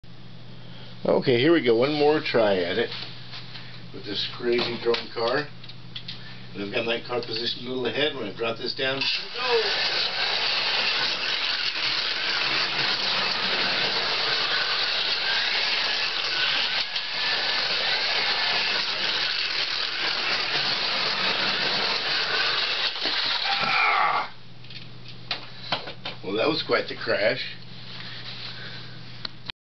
Crazy Fast Drone Car part Big 4 Crash on Back Straight (Carrera Go 1/43 Scale Slot Car Racing)
I'm still trying to beat this crazy fast drone car as demonstrated on an earlier video. I was at least keeping up for the first few laps this time but we were too much neck-and-neck, causing a big crash and slide at the lane change on the back straight.